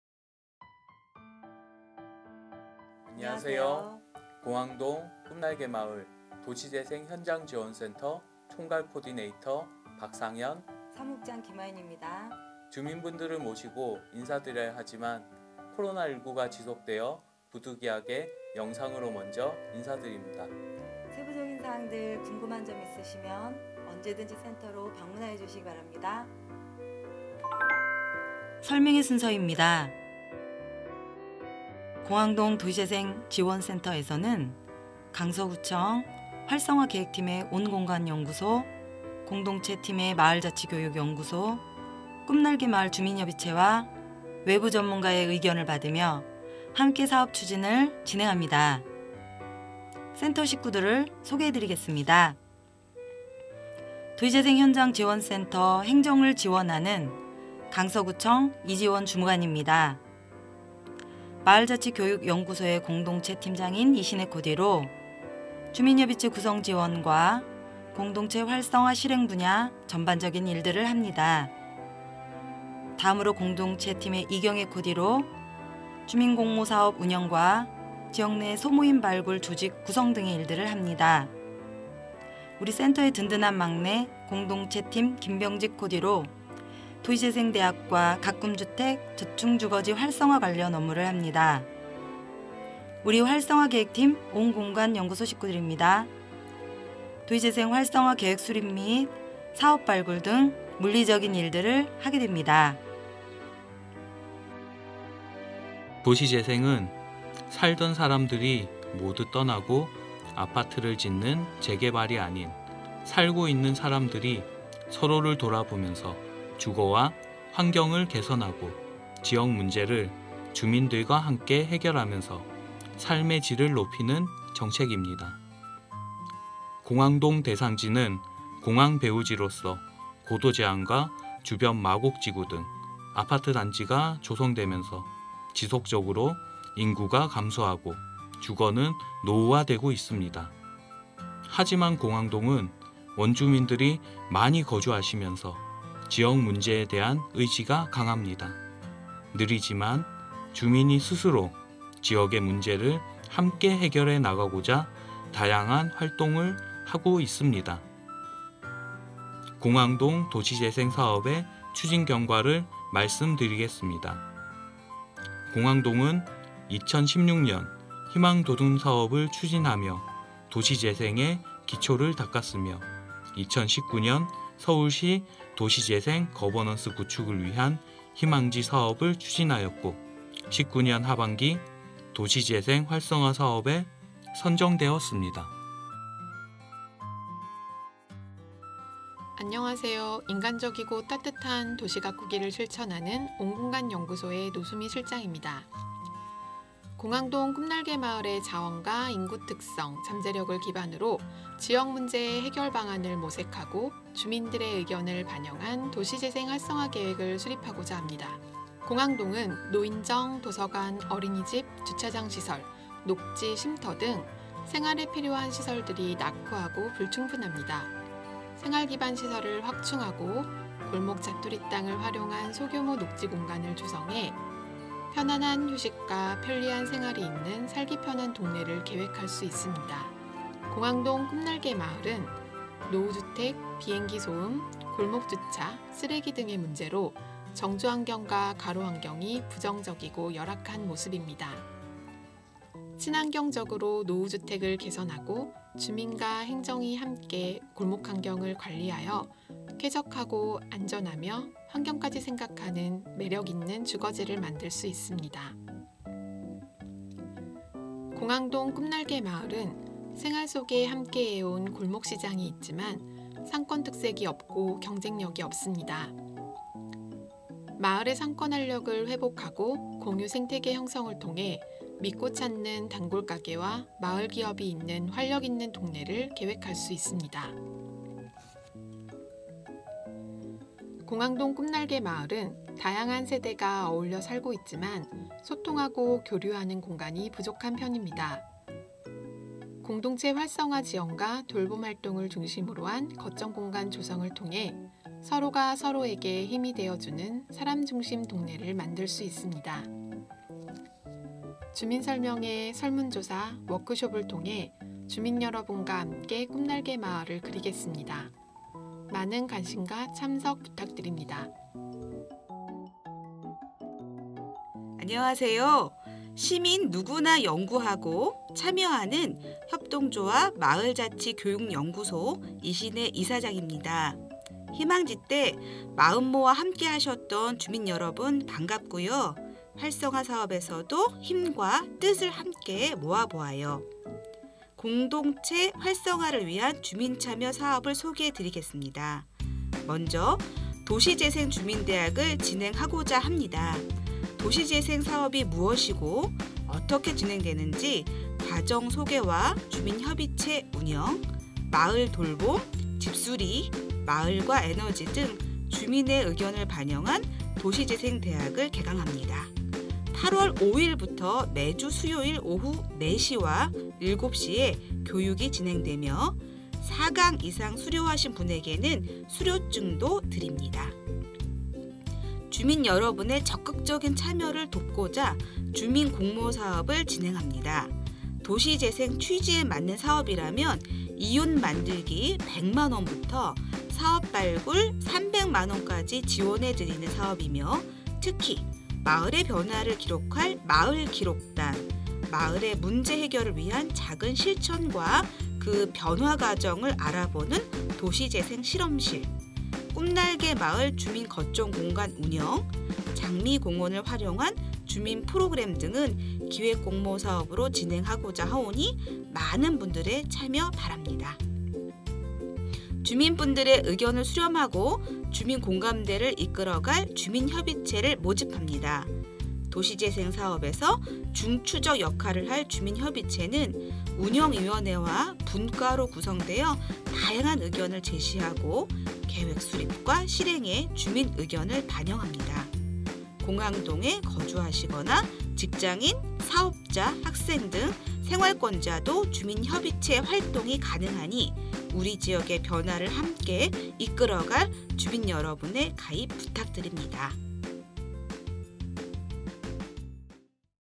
공항동 도시재생활성화계획 주민설명회
공항동도시재생지원센터에서는
강서구청, 활성화계획팀의 온공간연구소, 공동체팀의 마을자치연구소, 꿈날개마을 주민협의체와 외부전문가의 의견을 받으며 함께 사업추진을 진행합니다.

※ 서체는 마포서체(Mapo당인리발전소, Mapo꽃섬) 사용